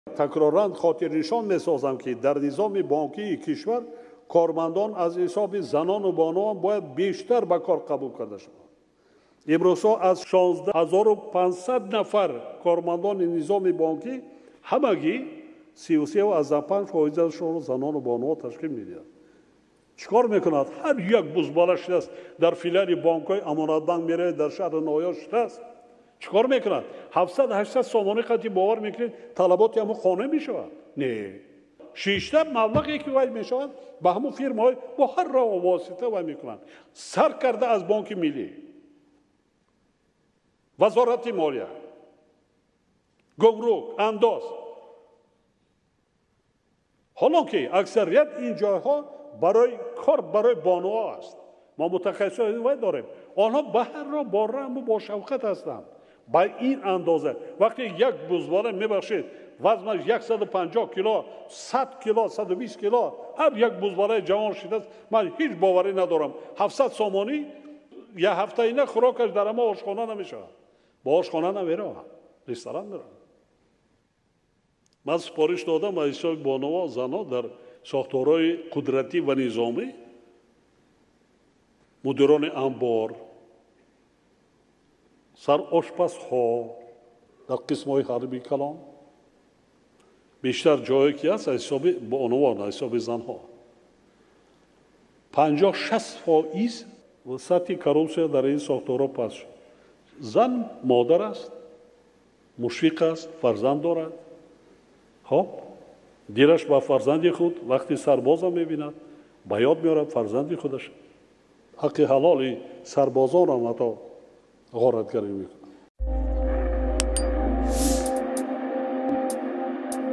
Рахмон: с зарплатой в 700 сомони ходят в рестораны
Президент Таджикистана призвал нанимать на работу в банках женщин. Эмомали Рахмон считает, что здоровым молодым парням не место в этой сфере: они зарабатывают на махинациях. А женщины милосердные и честные.